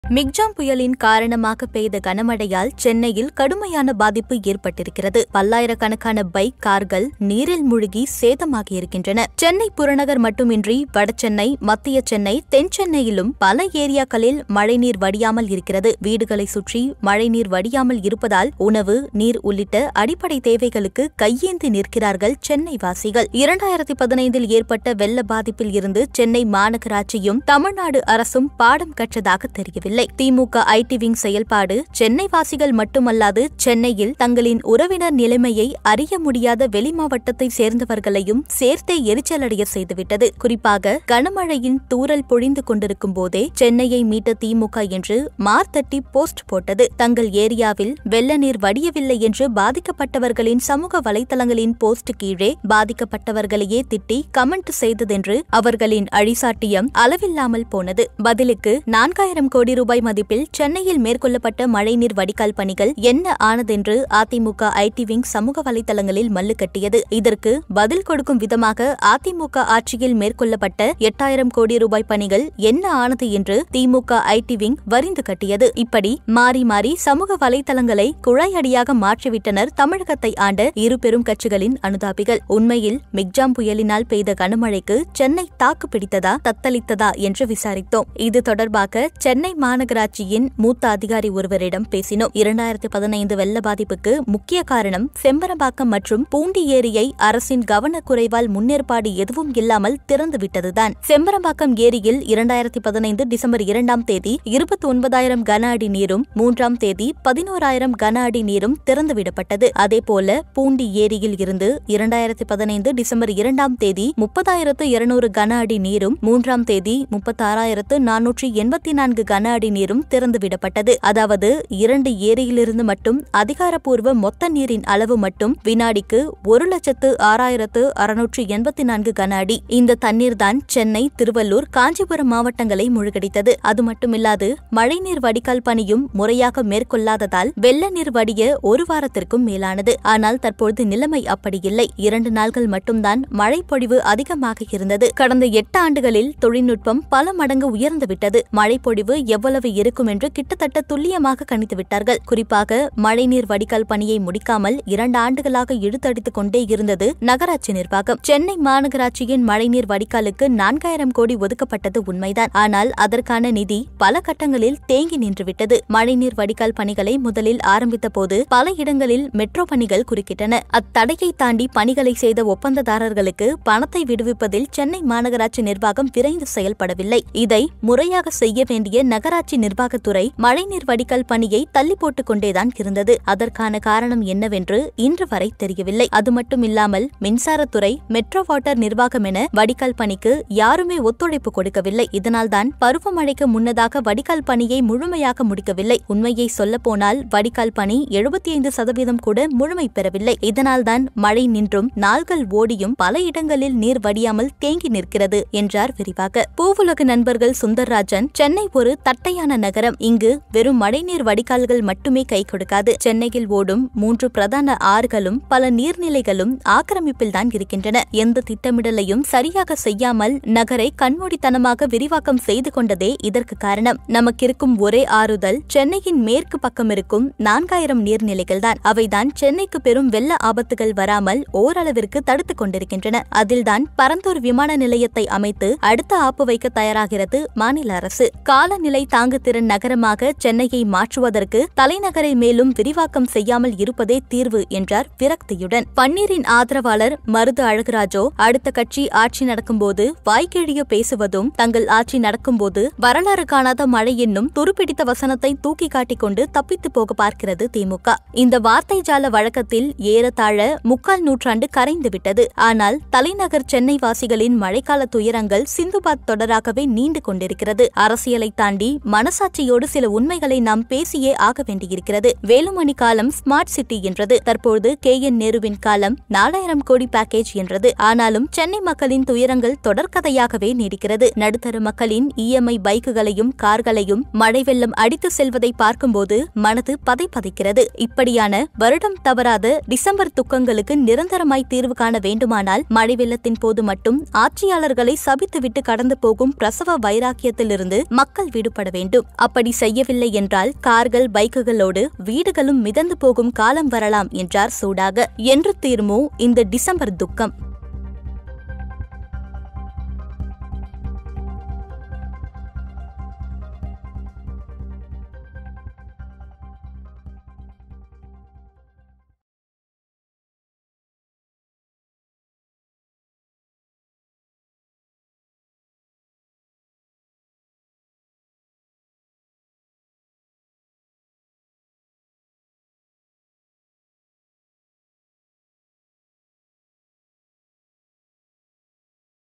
என்று தீருமோ இந்த `டிசம்பர்' துக்கம்?! | Vikatan
கடந்த அ.தி.மு.க ஆட்சியில் ஏற்பட்ட வெள்ள பாதிப்பின்போது, `ஸ்மார்ட் சிட்டி திட்டம் என்னாச்சு?’ என்று தி.மு.க கேள்வி எழுப்பியது. தற்போது தி.மு.க ஆட்சியில் வெள்ள பாதிப்பின்போது, `4,000 கோடி ரூபாய் என்னாச்சு?’ என்று அ.தி.மு.க கேள்வி எழுப்புகிறது.

மிக்ஜாம் புயலின் காரணமாக பெய்த கனமழையால், சென்னையில் கடுமையான பாதிப்பு ஏற்பட்டிருக்கிறது. பல்லாயிரக்கணக்கான பைக், கார்கள் நீரீல் மூழ்கி சேதமாகியிருக்கின்றன.

எழுத்து - மனோஜ் முத்தரசு | படத்தொகுப்பு - சிரஞ்சீவி